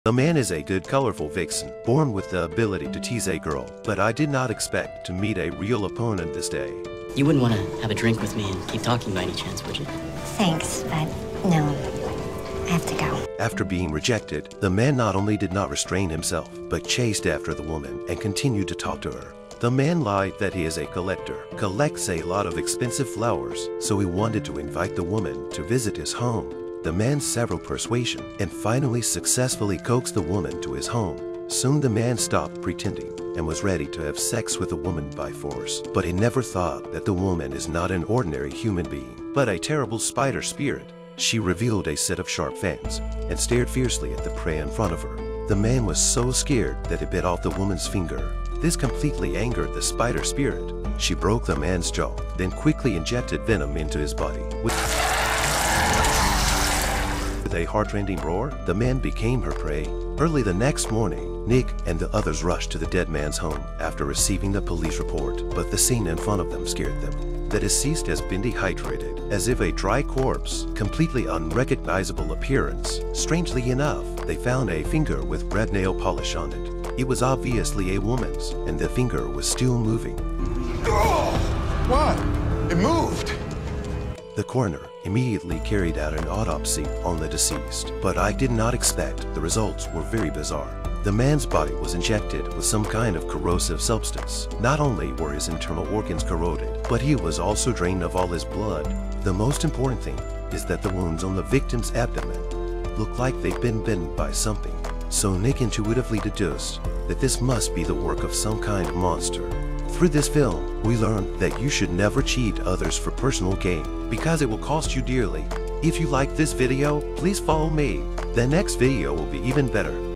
Vixen man lures woman to his home, not realising that women are not normal humans either
Brief:  The man was a vixen. On this day he was going to lure a woman to his house and wanted to have sex with her by force. Little did he realise that the woman was also a monster.